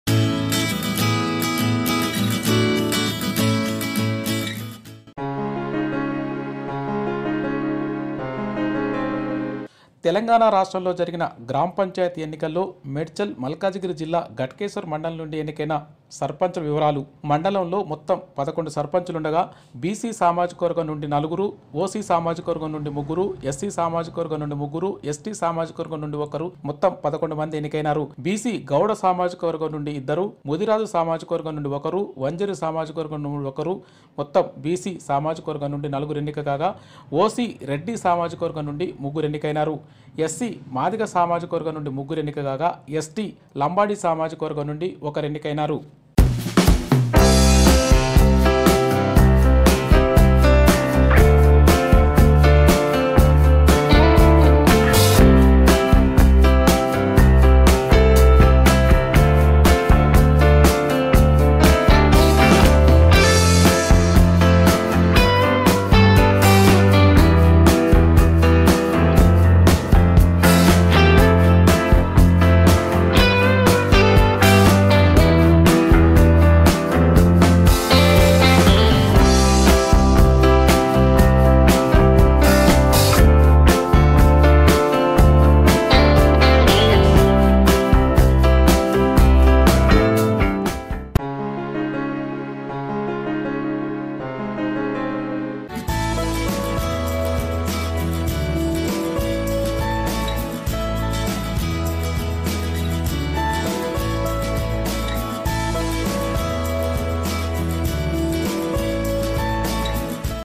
Medchal Malkajgiri Ghatkesar Mandal Sarpanches Profiles Caste Wise | TS Gram Panchayat Elections
Ankushapur Sarpanch Kommidi Jalaja
Aushapur Sarpanch Yenugu Kaveri Reddy
Chowdariguda Sarpanch Bairu Rama Devi (President)
Edulabad Sarpanch Kaleru Suresh
Ghanapur Sarpanch Nanavath Padma
Kachivanisingaram Sarpanch Kontham Venkatreddy
Korremula Sarpanch Ooruganti Venkatesh
Madharam Sarpanch Allola Yadagiri
Marpallyguda Sarpanch Chiluguri Mangamma
Pratapsingaram Sarpanch Vanguri Shivashankar
Venkatapur Sarpanch Nirdi Geetha